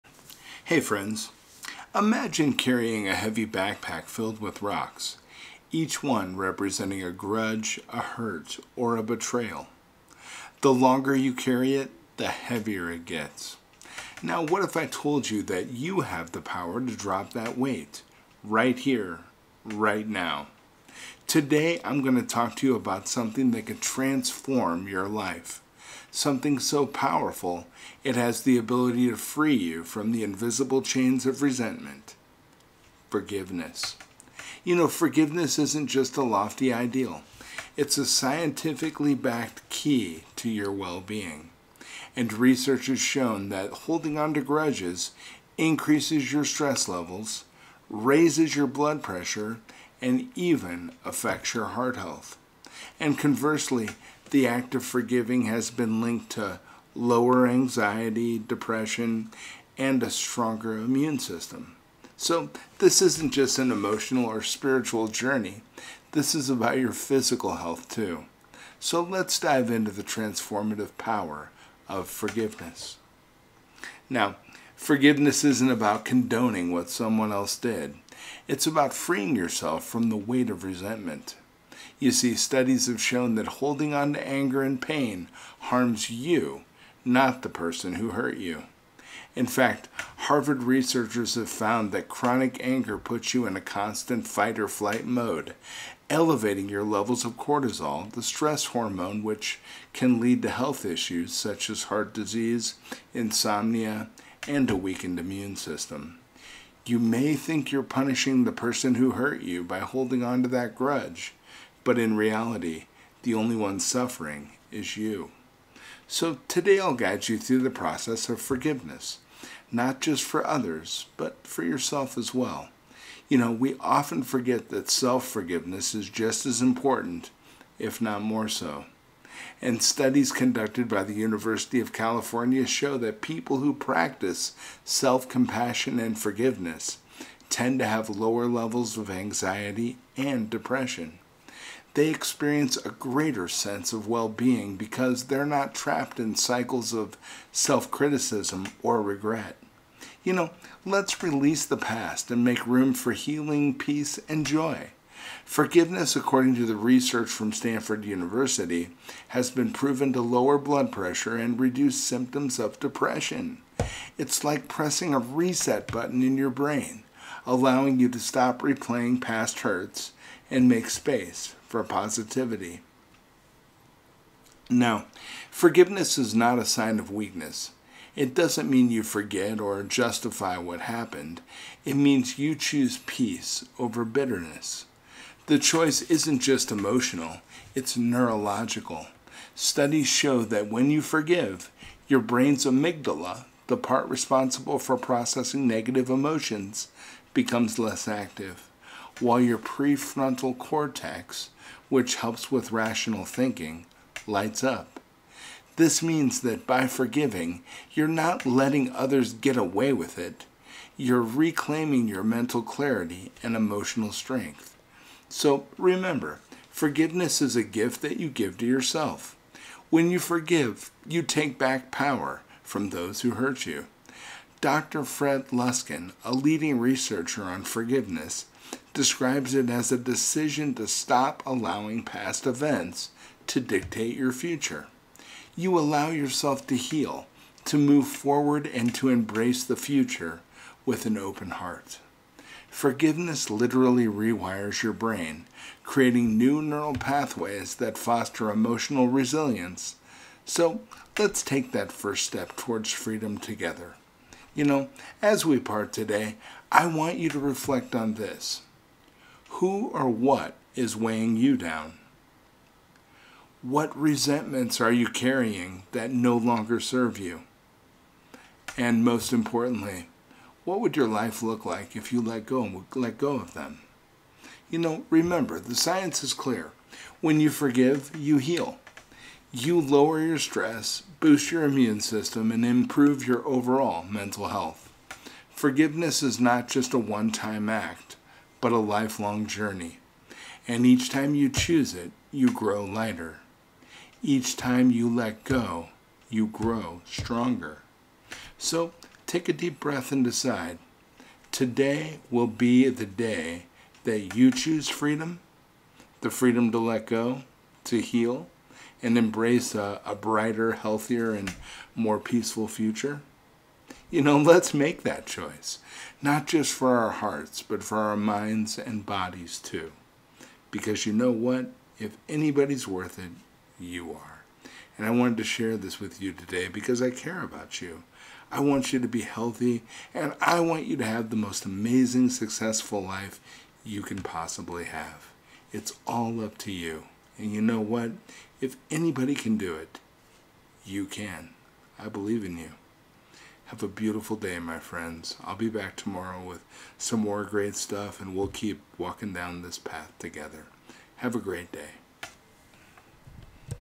How to Let Go of Resentment and Embrace Forgiveness | A Path to Inner Peace
Join Douglas Vandergraph as he dives into the transformative power of forgiveness and how to finally let go of resentment. In this video, Douglas shares practical strategies backed by science on how to practice forgiveness for yourself and others. Whether you're struggling with anger, pain, or past hurts, this video will guide you on a journey to emotional freedom and lasting peace. You'll learn how forgiveness can improve your mental health, reduce stress, and create stronger, healthier relationships. Don't let resentment control your life—start your healing journey today with these actionable steps!